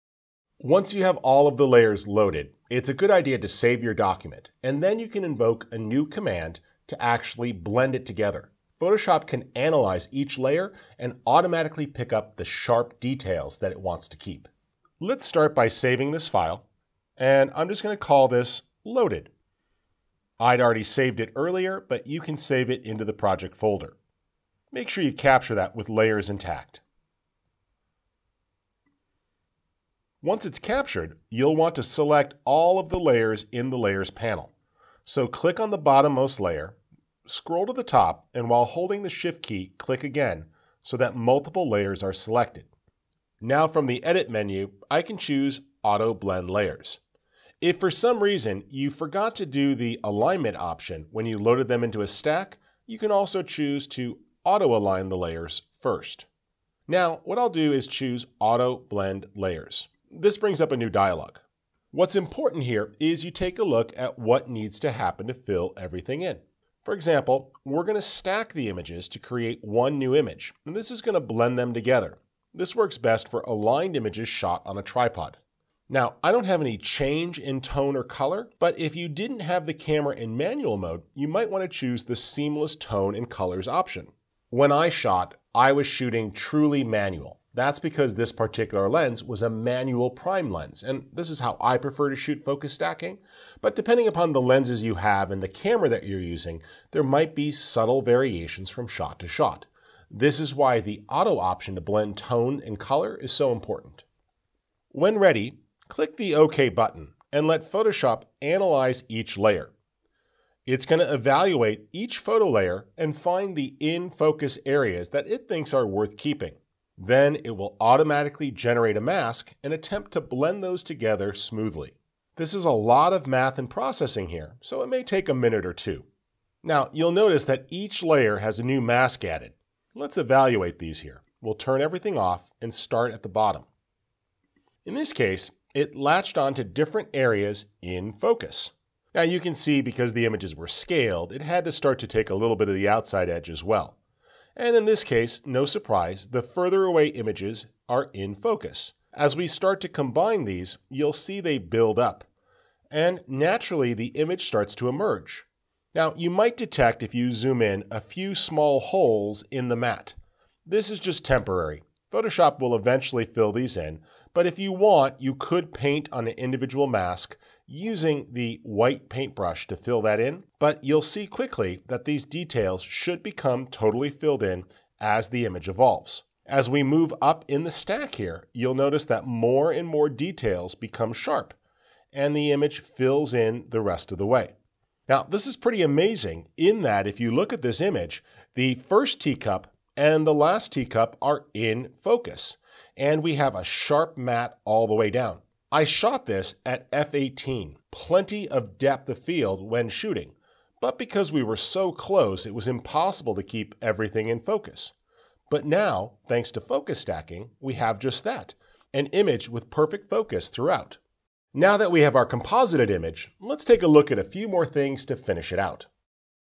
How to Blend Focus Stacked Images in Photoshop
How to Blend Focus Stacked Images in Photoshop | Step-by-Step Tutorial

Learn how to blend focus-stacked images in Photoshop to create stunning photos with perfect sharpness from front to back. In this easy-to-follow tutorial, we’ll guide you through the entire process—from loading your layers to using the Auto-Blend Layers feature for seamless results. Whether you’re shooting macro photography, landscapes, or product images, focus stacking is the secret to achieving maximum depth of field and professional-quality images.

✅ What You’ll Learn in This Photoshop Focus Stacking Tutorial:

How to load and align multiple focus-stacked images in Photoshop

Using Auto-Blend Layers to merge sharp areas automatically

When to use “Seamless Tones and Colors” for consistent blending

Tips for fixing small blending errors with masks and brushes

Why focus stacking is essential for macro and close-up photography

📸 Why Use Focus Stacking?
When shooting close-up subjects or scenes with extreme depth, it’s nearly impossible to keep everything in focus in a single shot. Focus stacking solves this by combining multiple images shot at different focus points into one perfectly sharp image.

💻 Tools Used in This Tutorial:

Adobe Photoshop (latest version)

Tripod and manual focus lens (recommended for best results)

🎥 Watch now and take your Photoshop skills to the next level!